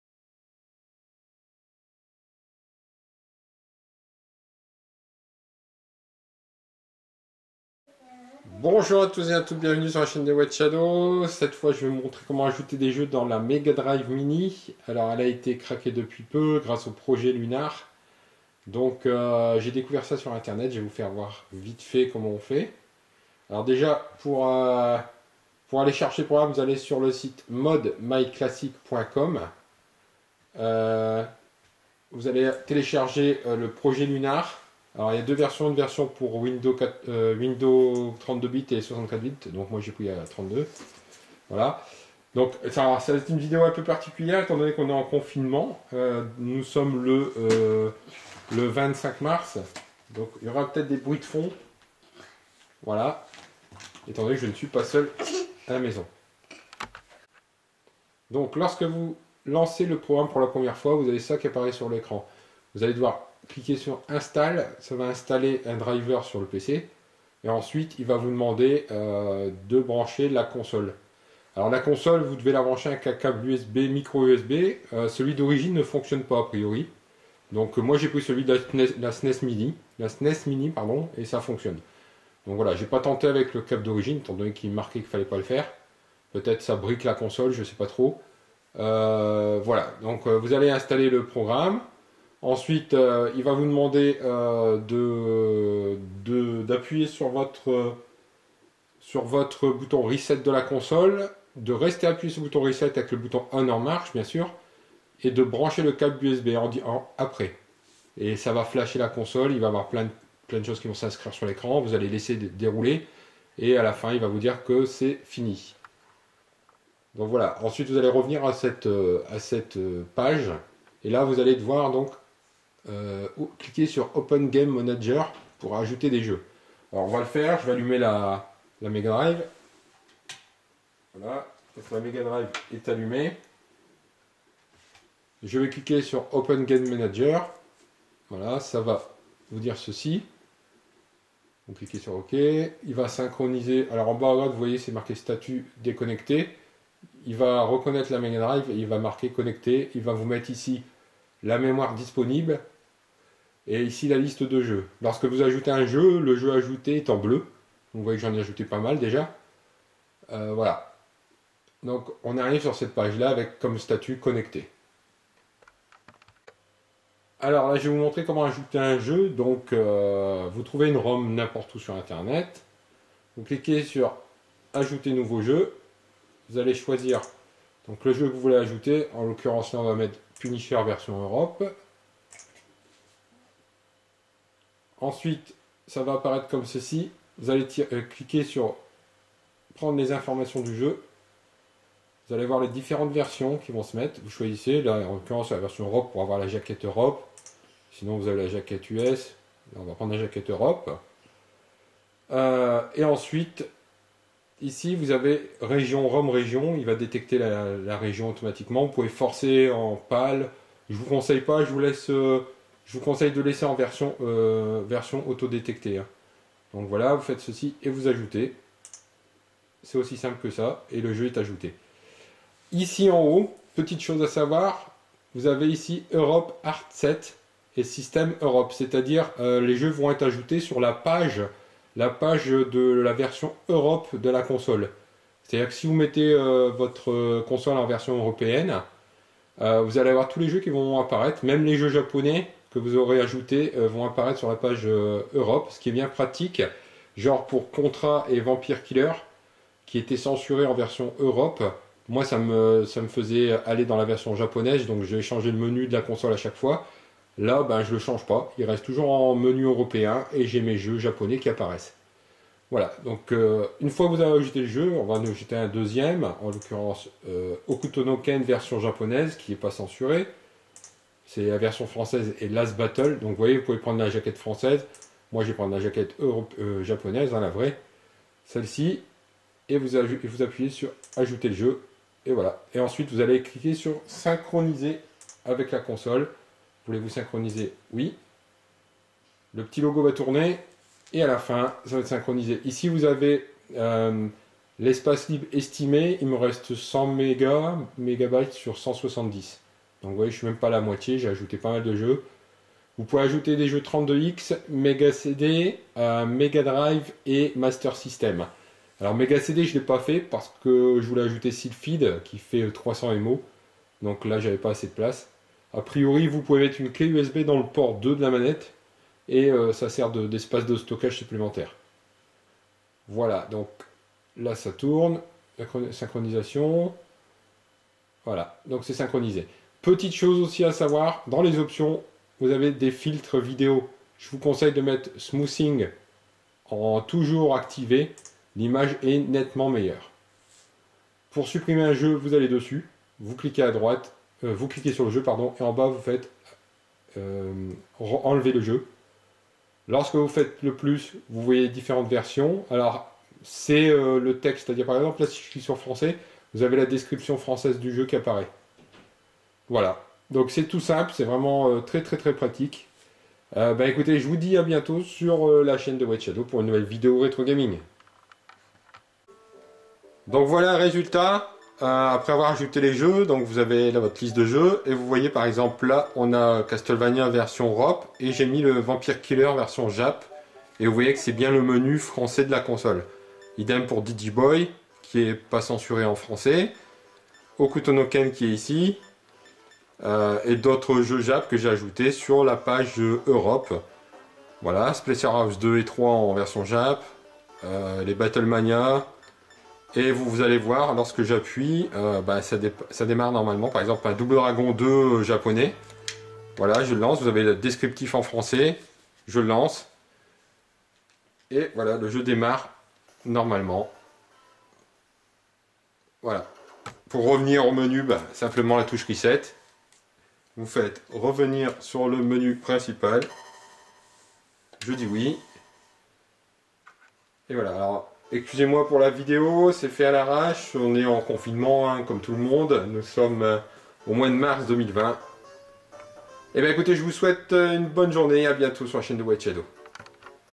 Comment Ajouter des jeux sur MEGADRIVE MINI (projet lunar)
il vous faudra le programme PROJET LUNAR (ici la version 1.05)